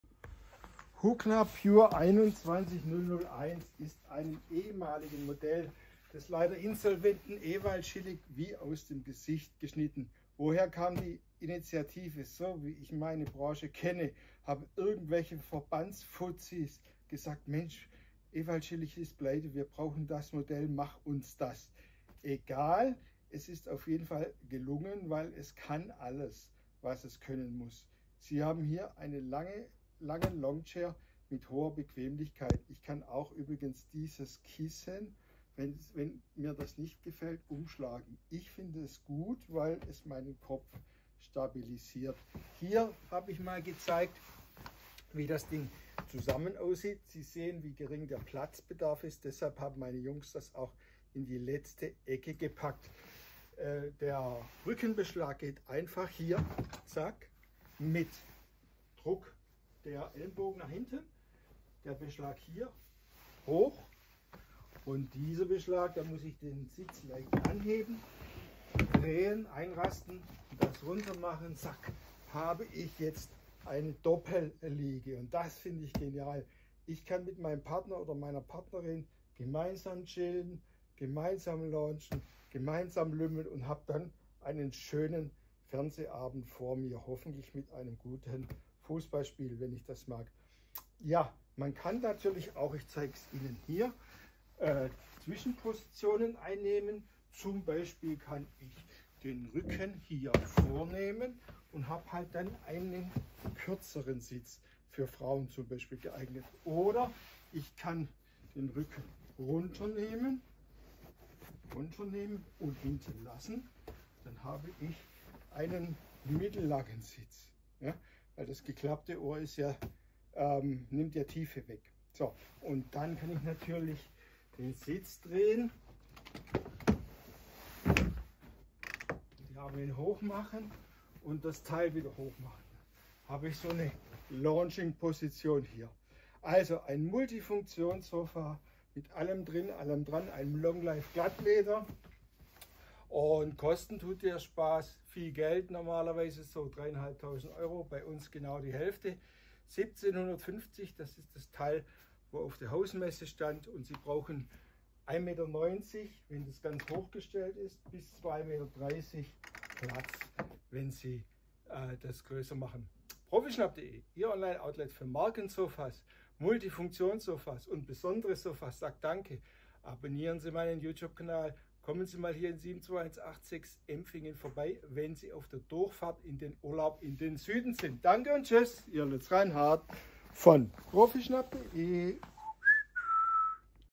Hukla Pure 21001 Sofa 190 cm 2 Drehsitze Rückenverstellung Leder Credo schwarz by profischnapp
Auf der Hausmesse von Hukla in der Kollektion ...
... Pure wurde dieses Sofa gezeigt und vorgestellt. Und dann? Dann kam das Sofa via 1b-Warenlager vom Hersteller zu uns. Die wohnliche Formgebung ist langlebig und unvergänglich, sie lässt sich mit vielen anderen Möbelstücken ideal kombinieren. Der ergonomische Sitzkomfort dieses Topmodelsbesticht durch einen echten Wohlfühl-Charakter. Mit 2 Drehsitzen, abklappbarer Armlehne und hochstellbaren und in der Tiefe vesrtellbaren Rücken inklusive. Relaxing pur durch die Drehfunktion wird hier auf einem Zweisitzer möglich und das auch noch für 2 Personen! Der Rindsnappe-Glattlederbezug in dieser klassichen Farbe ist hochwertig und pflegeleicht. Mit LCK-Lederpflegeset für Longlifeleder einfach zu reinigen.

Wenn Sie ein Multifunktionssofa in moderner Optik in einem robustem Leder suchen, sind Sie hier genau richtig. Das Modell Pure 21001 wird der Mittelpunkt Ihres Wohnzimmers.